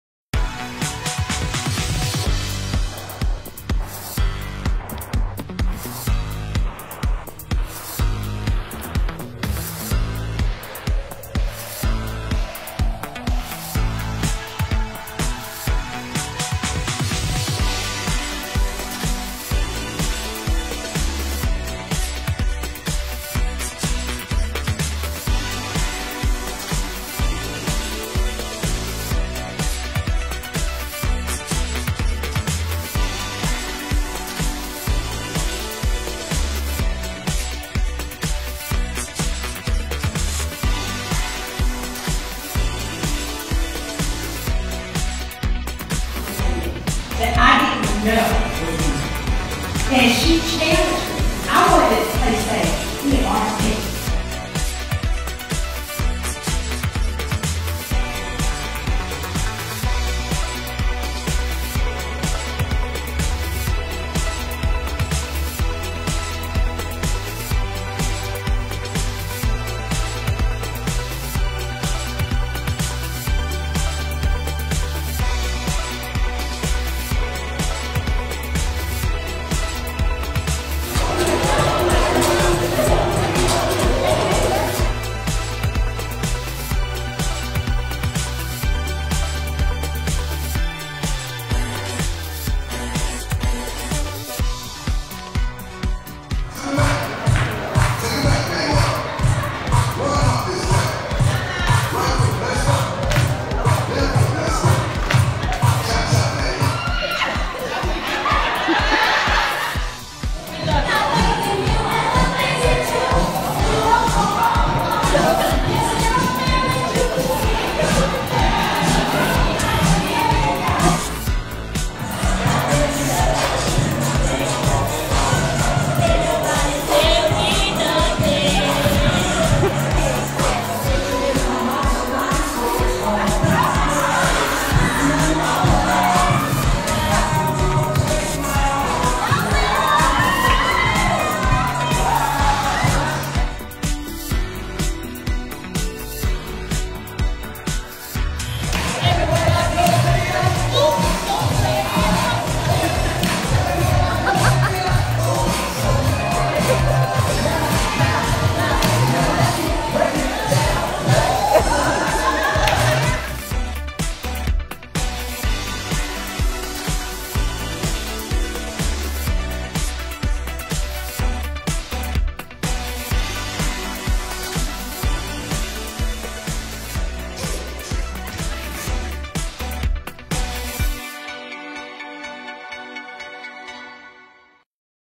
Annual Early Childhood Professional Appreciation Dinner - 2019
Last night we had the honor of celebrating the hardworking Early Childhood Professionals of Harnett County at our annual Appreciation Dinner. A wonderful night filled with great information by guest speaker, Ms. Tanya Dennis, as well as lots of smiles, laughter, doorprizes, and dancing!

Thank you for all you do, Harnett County Early Childhood Professionals!